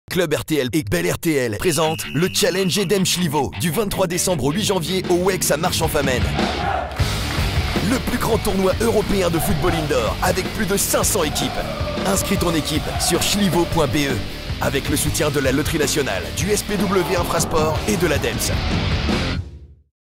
Challenge Edhem Sljivo 2016 - Spot TV
Du 23 décembre 2016 au 8 janvier 2017, participez au plus grand tournoi européen de football indoor, au WEX à Marche-en-Famenne !